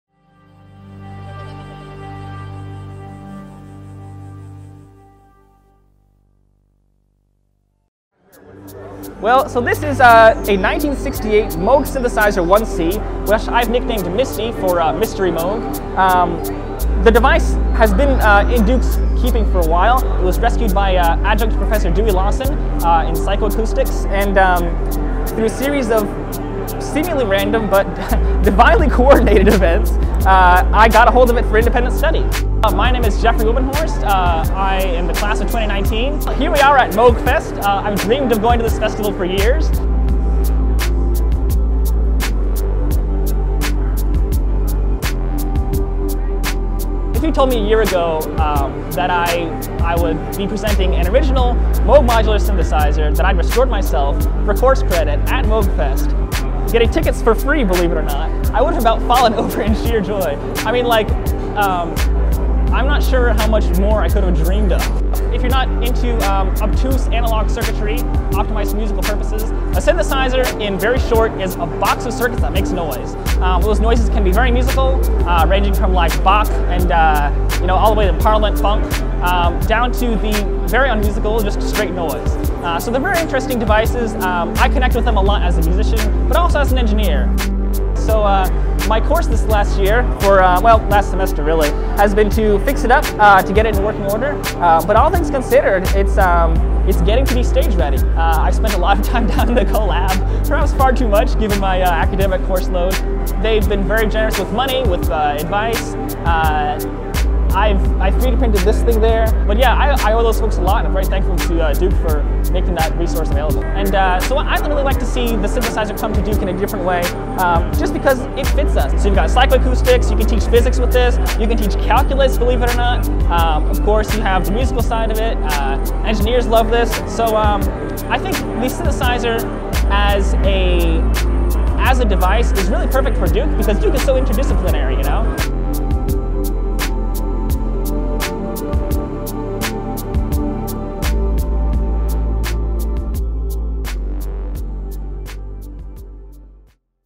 Duke Student Synthesizes Music & Machinery at Moogfest 2016
When Jeffrey Wubbenhorst (Class of 2019) began to restore a decommissioned synthesizer for his independent study, he wasn't sure what it would lead to. Fast forward a semester, and the Duke student and his refurbished Moog Synthesizer 1c -- affectionately nicknamed "Misty" or "Mystery Moog" -- have landed at Moogfest 2016: a mecca for engineers, futurists and electronic artists.

Wubbenhorst explains what a synthesizer is, how synthesizers draw upon several specialties ranging from calculus to psychoacoustics, and what the synthesizer could mean for interdisciplinary learning at Duke University. He also discusses his work with the Innovation Co-Lab, a creativity incubator at Duke.

Wubbenhorst is an undergraduate student in Duke University's Pratt School of Engineering, where he studies electrical engineering, computer science and music. He aspires to start an analog synth lab on campus.

Moogfest is annual celebration of the life of Robert Moog, inventor and synthesizer pioneer. The event draws artists, entrepreneurs, futurist thinkers, musicians and engineers. In May 2016, the festival relocated to Durham, NC.